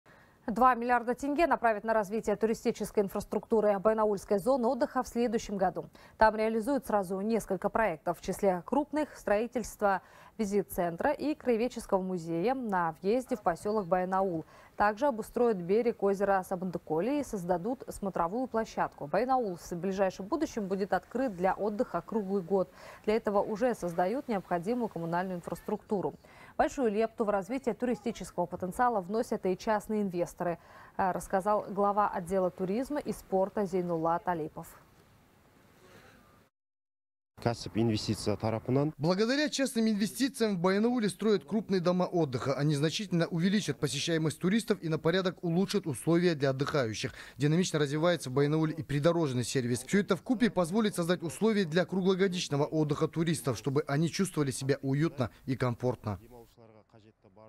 Два миллиарда тенге направят на развитие туризма в Баянауле
Два миллиарда тенге направят на развитие туристической инфраструктуры Баянаульской зоны отдыха в следующем году, передаёт «24KZ». Там реализуют сразу несколько проектов.